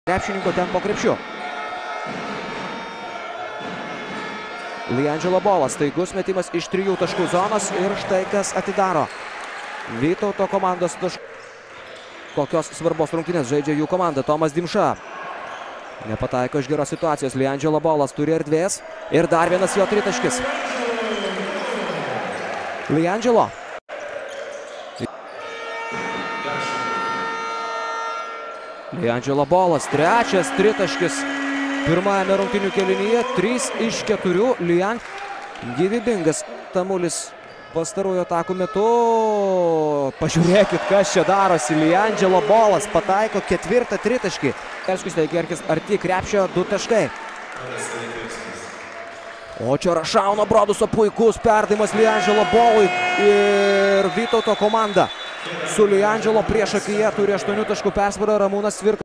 LiAngelo Ball starts the most important game of the season with 14 points in 1st QT
LiAngelo Ball records season-high 28 points (6/8 3pt) but Vytautas loses 77-90 to Nevezis Kedainiai and stays at the bottom of the league. Box-score